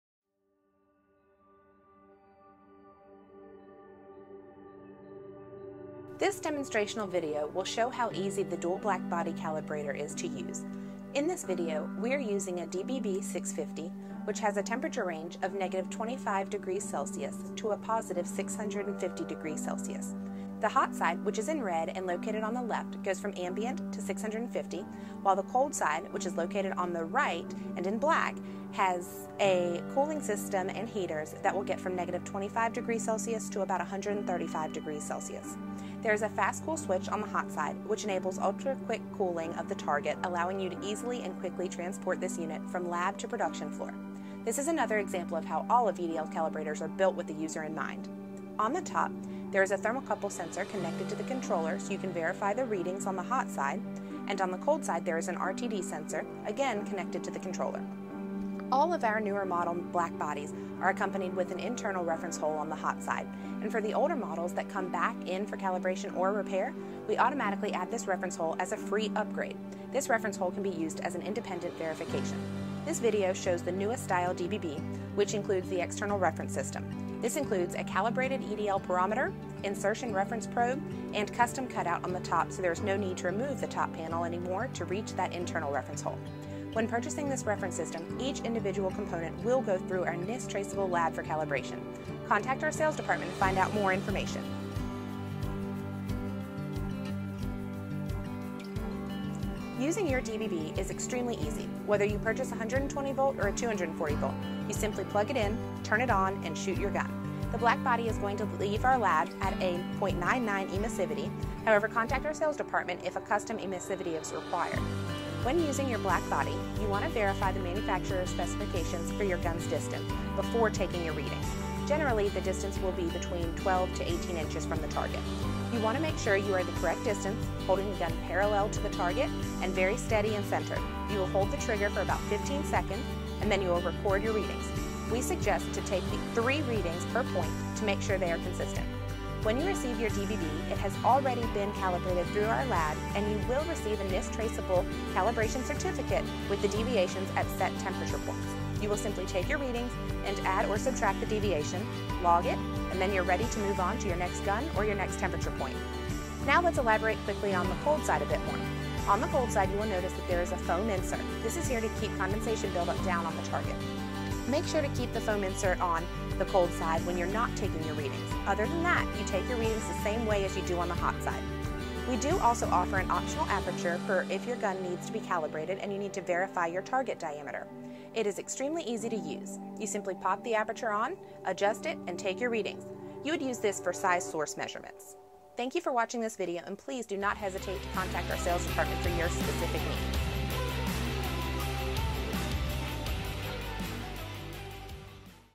EDL's Dual Black Body Infrared Calibrator (DBB-650)
Watch just how easy EDL's calibrators are to use! Watch this video to learn about the Dual Black Body, DBB-650 Calibrator.